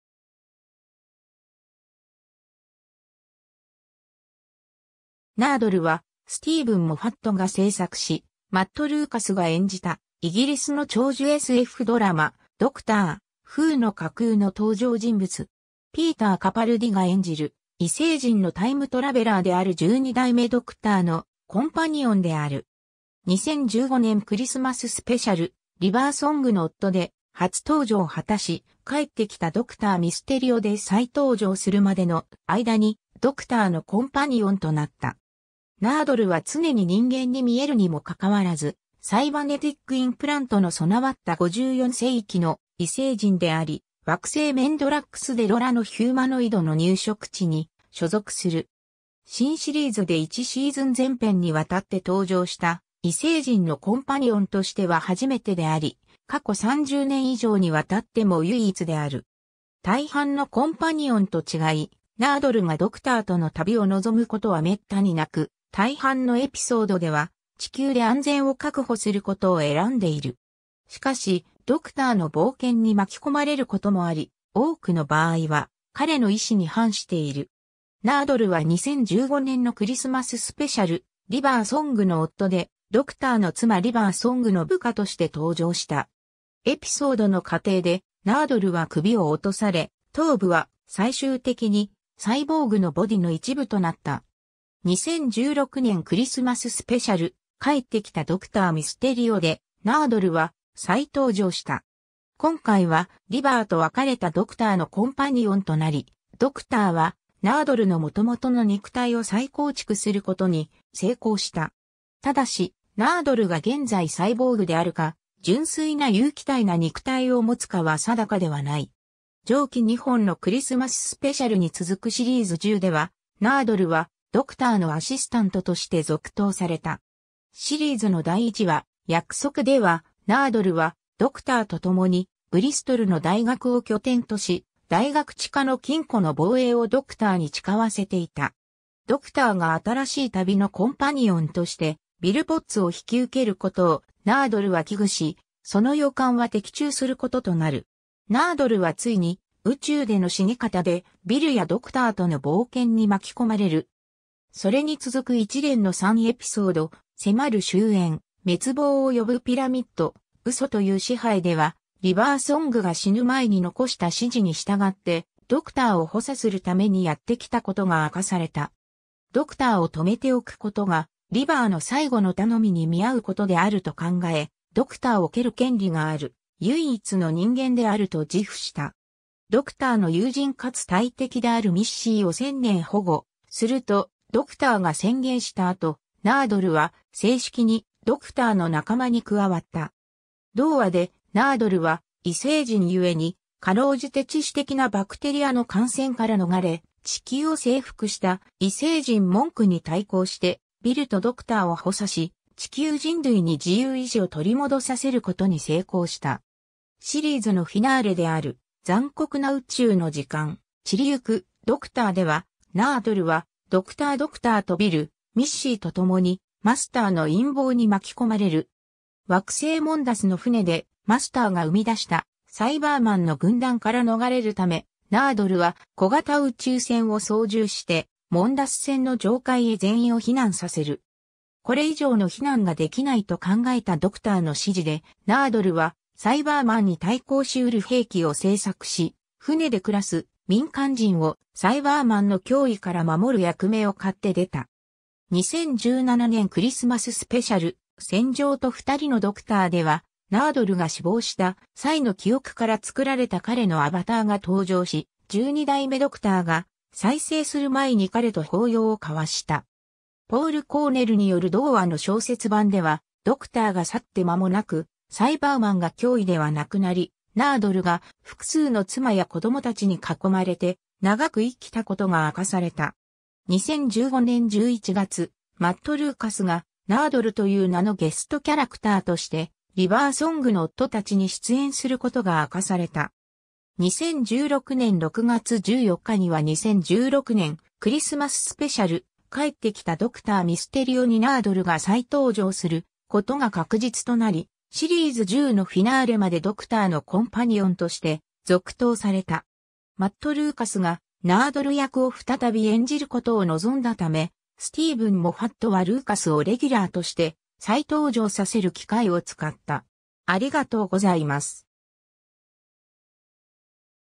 ナードル
ドクター・フーの異星人
ナードル（英: Nardole）は、スティーヴン・モファットが制作し、マット・ルーカスが演じた、イギリスの長寿SFドラマ『ドクター・フー』の架空の登場人物。
ピーター・カパルディが演じる異星人のタイムトラベラーである12代目ドクターのコンパニオンである。
2015年クリスマススペシャル「リヴァーソングの夫」で初登場を果たし、「帰ってきたドクター・ミステリオ」で再登場するまでの間にドクターのコンパニオンとなった。
 ナードルは常に人間に見えるにも拘わらず、サイバネティックインプラントの備わった54世紀の異星人であり、惑星メンドラックス・デロラのヒューマノイドの入植地に所属する。
新シリーズで1シーズン全編に亘って登場した異星人のコンパニオンとしては初めてであり、過去30年以上に亘っても唯一である。
大半のコンパニオンと違い、ナードルがドクターとの旅を望むことは滅多になく、大半のエピソードでは地球で安全を確保することを選んでいる。
しかし、ドクターの冒険に巻き込まれることもあり、多くの場合は彼の意思に反している。
 ナードルは2015年のクリスマススペシャル「リヴァー・ソングの夫」でドクターの妻リヴァー・ソングの部下として登場した。
エピソードの過程でナードルは首を落とされ、頭部は最終的にサイボーグのボディの一部となった。
2016年クリスマススペシャル「帰ってきたドクター・ミステリオ」でナードルは再登場した。
今回はリヴァーと別れたドクターのコンパニオンとなり、ドクターはナードルの元々の肉体を再構築することに成功した。
ただし、ナードルが現在サイボーグであるか、純粋な有機体な肉体を持つかは定かではない。
 上記2本のクリスマススペシャルに続くシリーズ10では、ナードルはドクターのアシスタントとして続投された。
シリーズの第1話「約束」では、ナードルはドクターと共にブリストルの大学を拠点とし、大学地下の金庫の防衛をドクターに誓わせていた。
ドクターが新しい旅のコンパニオンとしてビル・ポッツを引き受けることをナードルは危惧し、その予感は的中することとなる。
ナードルはついに「宇宙での死に方」でビルやドクターとの冒険に巻き込まれる。
それに続く一連の3エピソード「迫る終焉」、「滅亡を呼ぶピラミッド」、「嘘という支配」では、リヴァー・ソングが死ぬ前に残した指示に従ってドクターを補佐するためにやって来たことが明かされた。
ドクターを留めておくことがリヴァーの最期の頼みに見合うことであると考え、ドクターを蹴る権利がある唯一の人間であると自負した。
ドクターの友人かつ大敵であるミッシーを1000年保護するとドクターが宣言した後、ナードルは正式にドクターの仲間に加わった。
同話でナードルは異星人ゆえに辛うじて致死的なバクテリアの感染から逃れ、地球を征服した異星人モンクに対抗してビルとドクターを補佐し、地球人類に自由意志を取り戻させることに成功した。
 シリーズのフィナーレである「残酷な宇宙の時間」/「散りゆくドクター」では、ナードルはドクタードクターとビル、ミッシーと共にマスターの陰謀に巻き込まれる。
惑星モンダスの船でマスターが生み出したサイバーマンの軍団から逃れるため、ナードルは小型宇宙船を操縦してモンダス船の上階へ全員を避難させる。
これ以上の避難ができないと考えたドクターの指示でナードルはサイバーマンに対抗しうる兵器を製作し、船で暮らす民間人をサイバーマンの脅威から守る役目を買って出た。
 2017年クリスマススペシャル「戦場と二人のドクター」では、ナードルが死亡した際の記憶から作られた彼のアバターが登場し、12代目ドクターが再生する前に彼と抱擁を交わした。
ポール・コーネルによる同話の小説版では、ドクターが去って間もなくサイバーマンが脅威ではなくなり、ナードルが複数の妻や子どもたちに囲まれて長く生きたことが明かされた。
 2015年11月、マット・ルーカスがナードルという名のゲストキャラクターとして「リヴァー・ソングの夫たち」に出演することが明かされた。
2016年6月14日には2016年クリスマススペシャル「帰ってきたドクター・ミステリオ」にナードルが再登場することが確実となり、シリーズ10のフィナーレまでドクターのコンパニオンとして続投された。
マット・ルーカスがナードル役を再び演じることを望んだため、スティーヴン・モファットはルーカスをレギュラーとして再登場させる機会を使った。